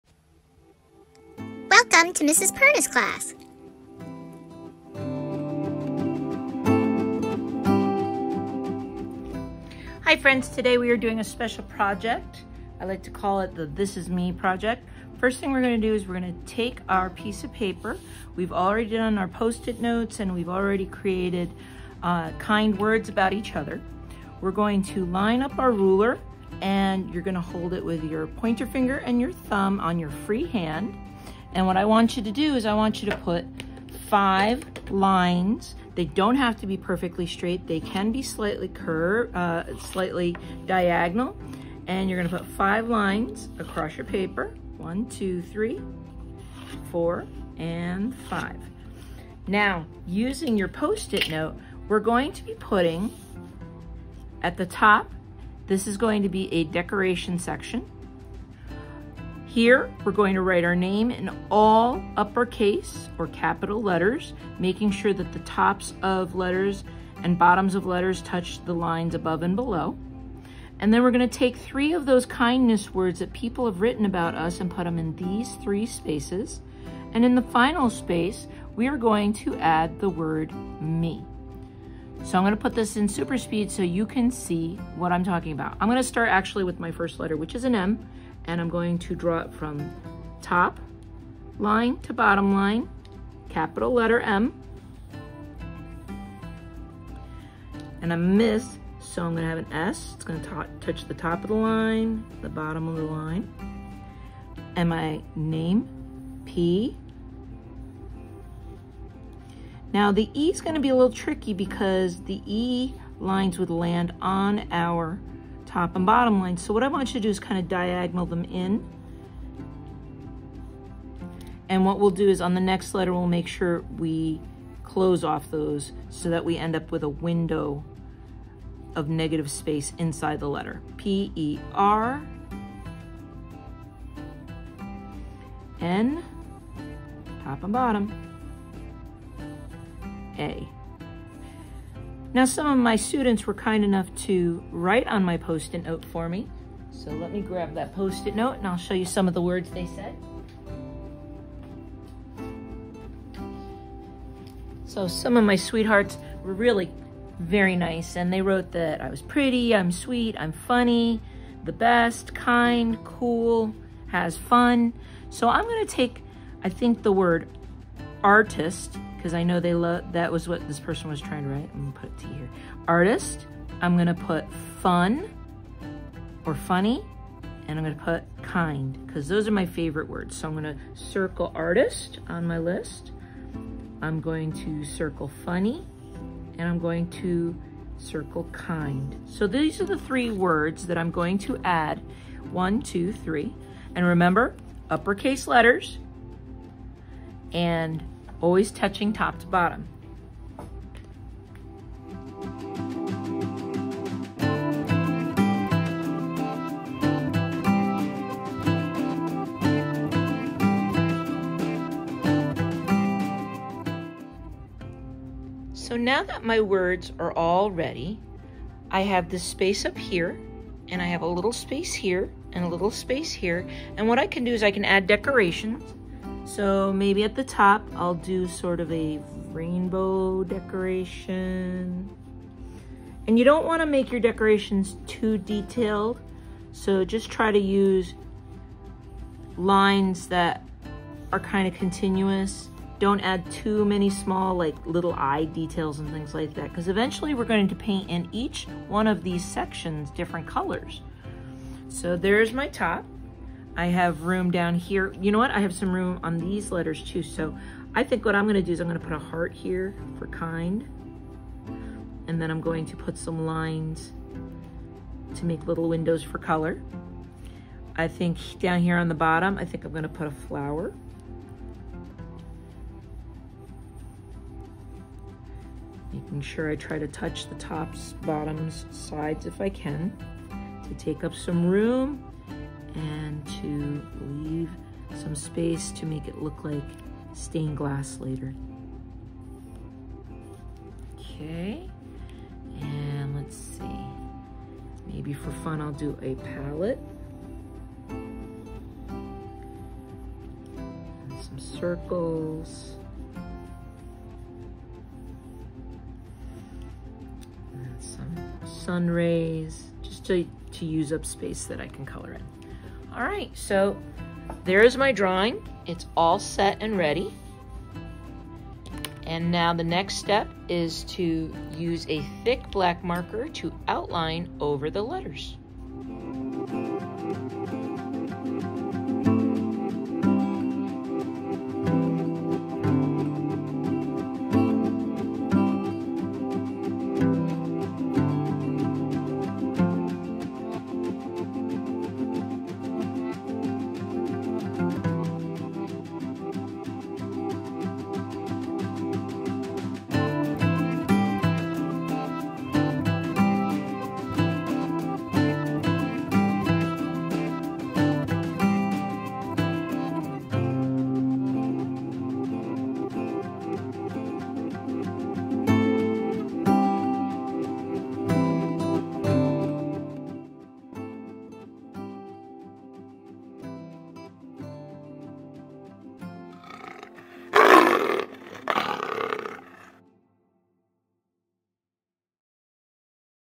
SEL: Me Painting Project part 1
In today’s video, we will be setting up our watercolor me painting. The students will generate words to describe themselves and using up letters create a Stainglass window effect for a watercolor painting. This video is part one of this project.